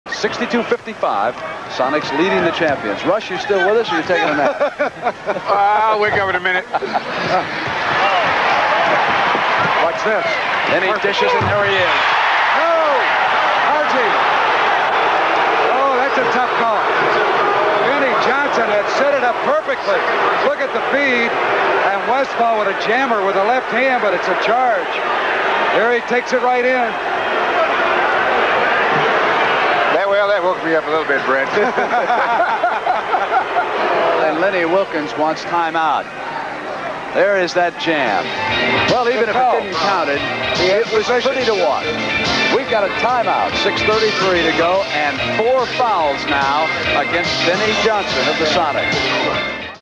Paul Westphal Posterizes Magic Johnson (Opening Night - 1980)
October 10, 1980 - In his first game since a 42-point, 15-rebound, 7-assist championship-clinching rookie performance against the Philadelphia 76ers five months earlier, Magic Johnson received a rude awakening from Paul Westphal, who served up a facial midway through his Seattle SuperSonics debut. Westphal finished with 24 points but the Sonics were ultimately doomed by a Michael Cooper game-winner, which can be seen

Johnson finished with 14 points, 11 rebounds, and 8 assists.

Commentary: Brent Musburger, Bill Russell, Hot Rod Hundley (CBS)